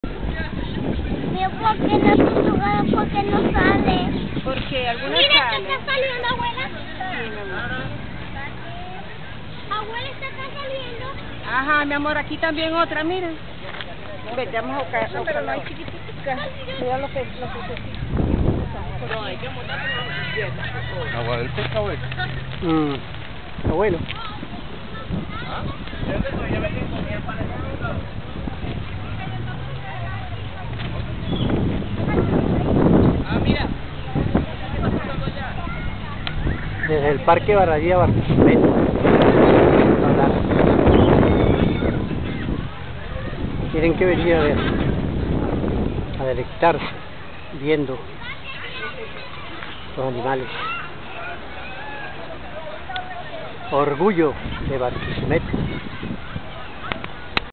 PICT0057 VISITA FAMILIAR,.
UNA VISITA FAMILIAR A EL PARQUE BARADIDA,,LLENA DE AMOR,CARIÑO,JUNTO A MIS HIJOS Y NIETOS,.QUE U PUEDE HACER,JUNTO A SUS FAMILIARES,Y AMIGOS ,CUIDANDO EL PARQUE,Y PONIENDO UN GRANITO DE ARENA,ES NUESTRO DEBER,SIN EGOÍSMO NI MEZQUINDADES,POR,LARA,Y PARA LARA,PODEMOS HACER MUCHO MAS,.DIOS BENDIGA A LARA,.